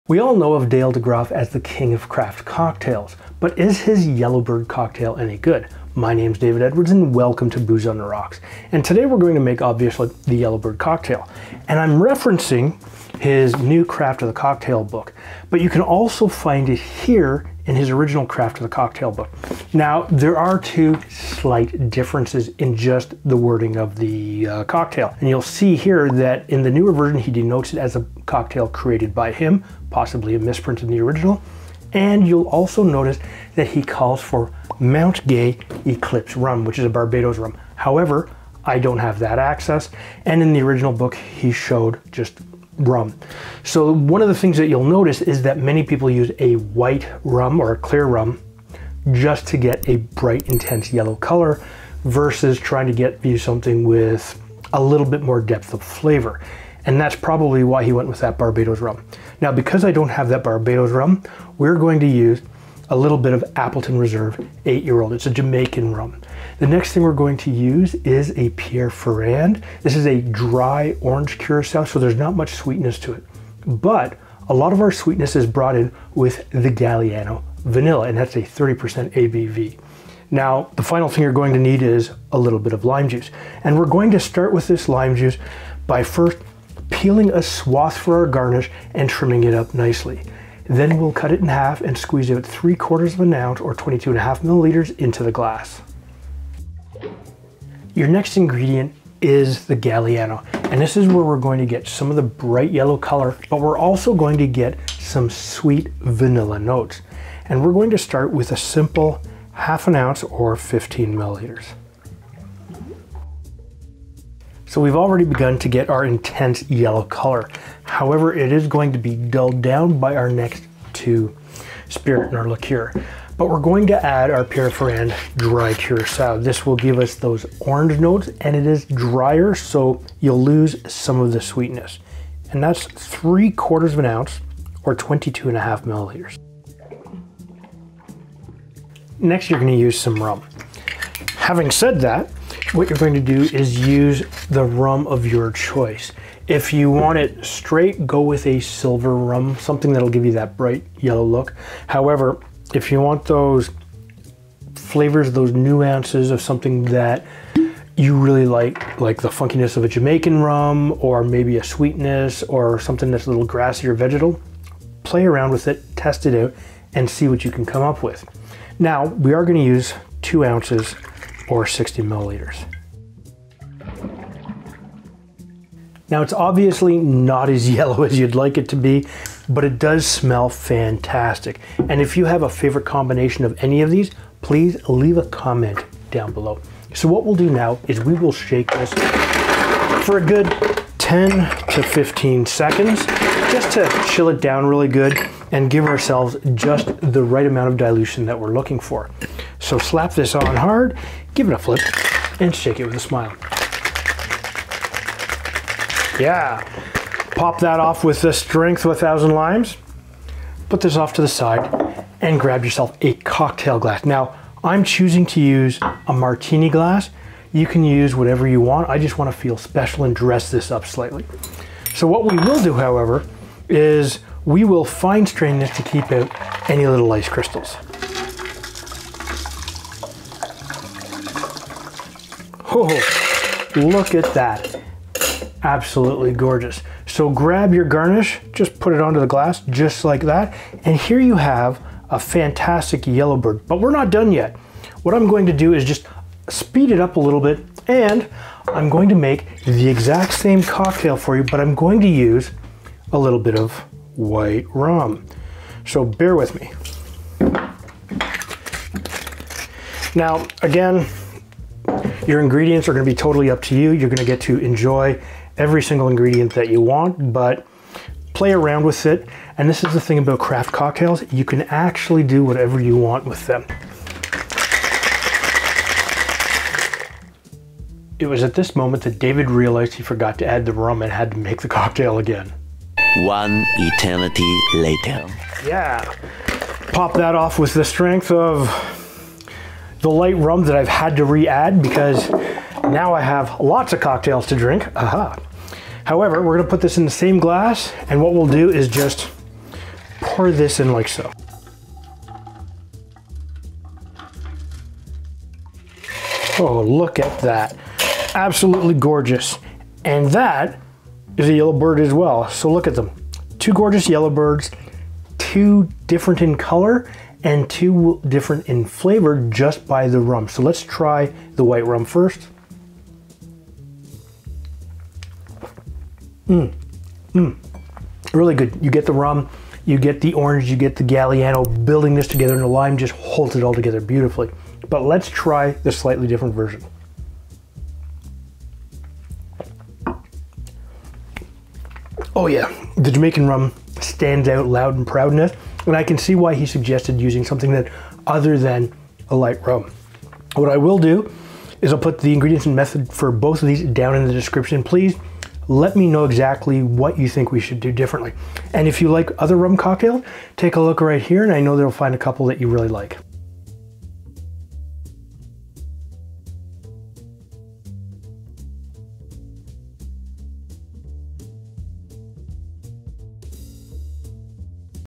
How To Make Dale Degroeff's Yellow Bird Cocktail
Today on Booze On The Rocks, we make a Yellow Bird cocktail. This cocktail is noted in the New Craft of The Cocktail by Dale Degreoff as an original creation. Interestingly, between the original Craft of The Cocktail and the revised edition. Dale suggests using Mount Gay Eclipse Rum in the new edition. However, I used Appleton Reserve 8. with the Galiano Vanilla and Dry Curacao, which gives it a light sweetness with orange notes, with the rum standing proud. This cocktail is an excellent sipper for after-dinner. Try it out, and let me know what you think.

BAR TOOLS

YELLOW BIRD

2 oz/60 ml Jamaican RUM
.75 oz/22.5 ml Orange Curacao
.5 oz/15 ml Galliano Vanilla
.75 oz/22.5 ml Lime Juice

Method

1. Add all of the ingredients to the shaker tin.
2. Shake with ice for 10-15 seconds.
3. Pour into a Martini glass.
4. Garnish with a Lime twist.